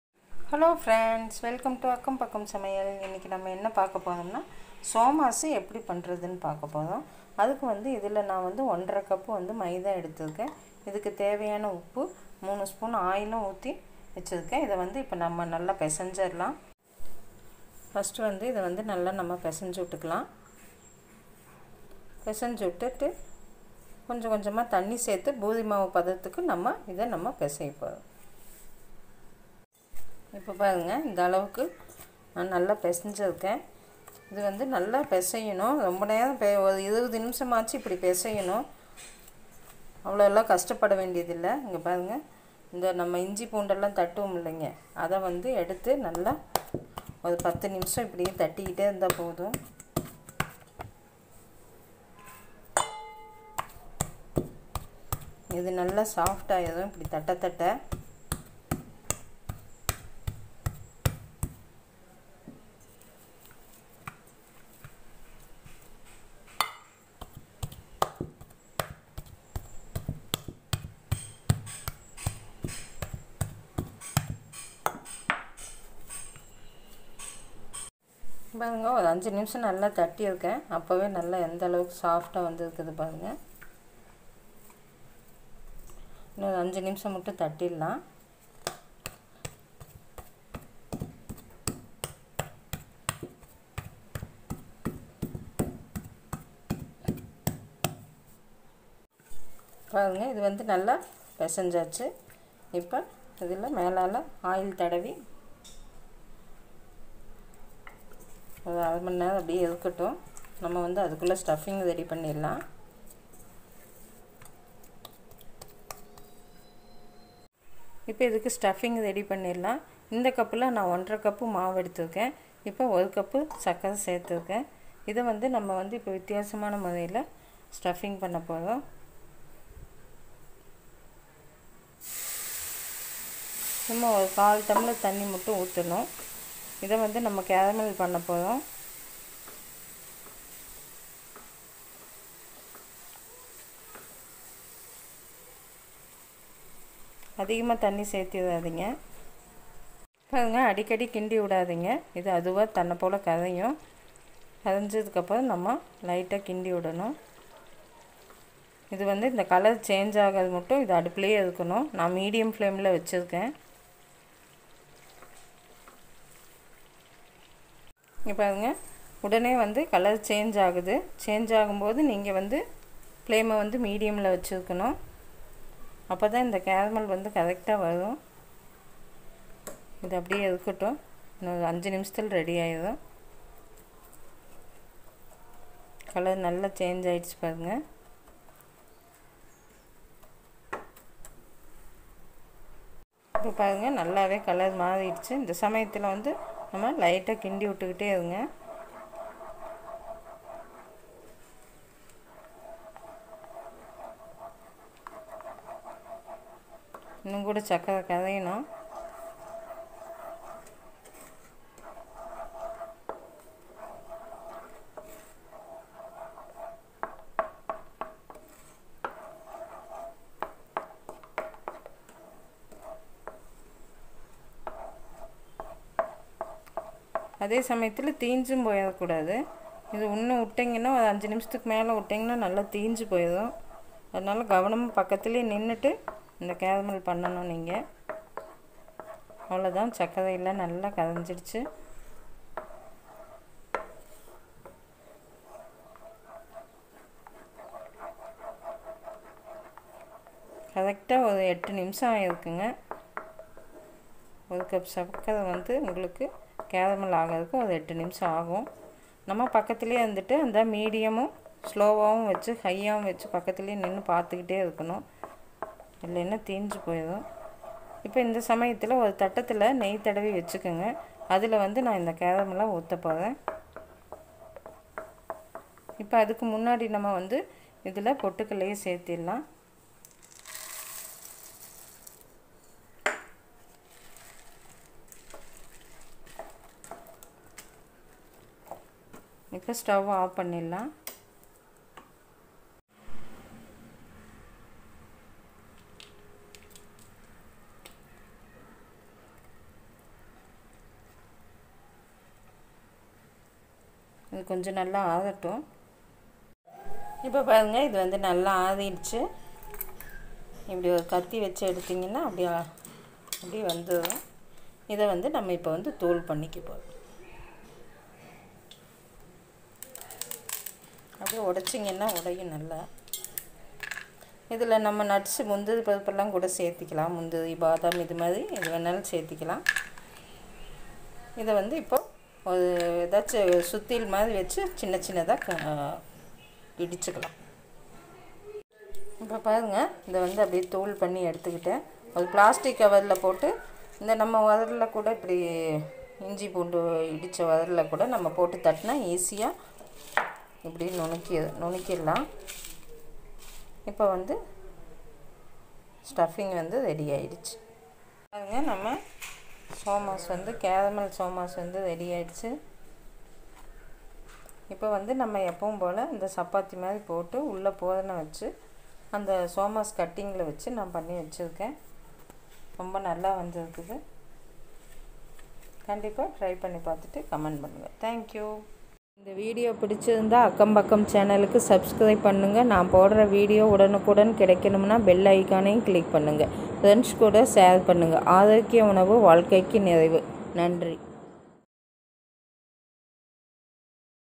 Somasu(Innovative Stuffing)in Tamil|| சோமாஸ்(புதுமையான பூரணம்)111
Maidha-1/4kg(11/2cup)
Sugar -1cup
Bengal gram -3/4cup(பொட்டுக்கடலை)